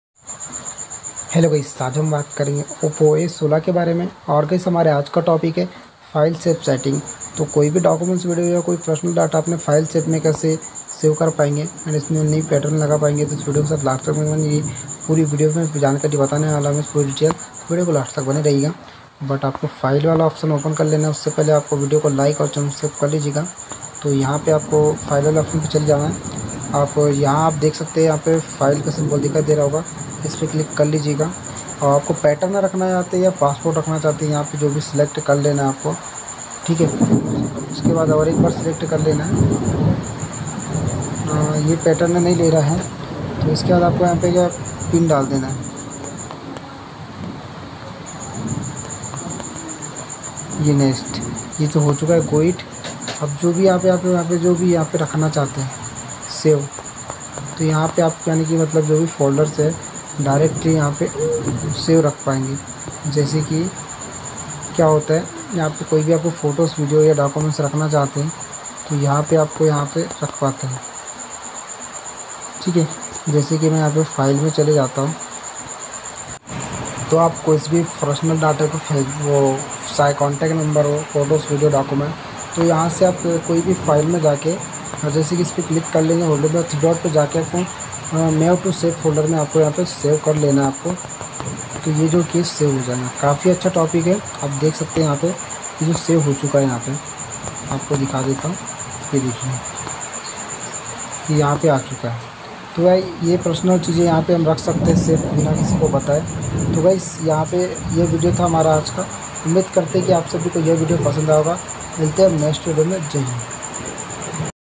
oppo A16 How to use file safe setting tips and tricks
hello guys es video me maine app ko file safe setting ke bhare me bataya hu ki kayse use kare ummid hai app ko video pasand aaya hoga
dhanyawad